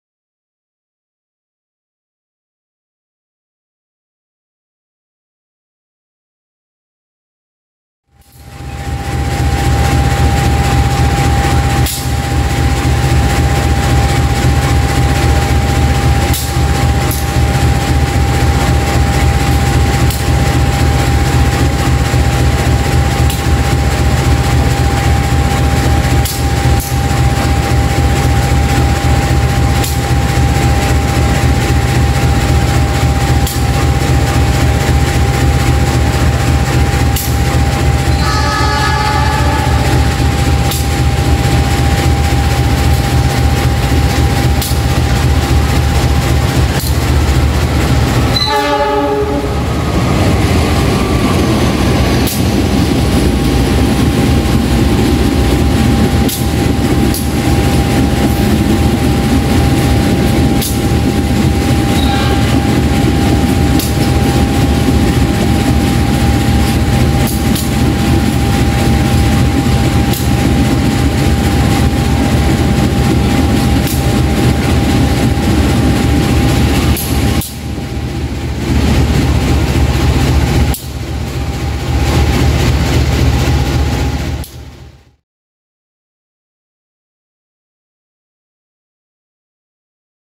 KA Argo Dwipangga Menyusul KA Bengawan di stasiun Lempuyangan
Assalamu'alaikum Viewers...

Berikut merupakan dokumentasi proses persusulan KA Argo Dwipangga dan KA Bengawan di stasiun Lempuyangan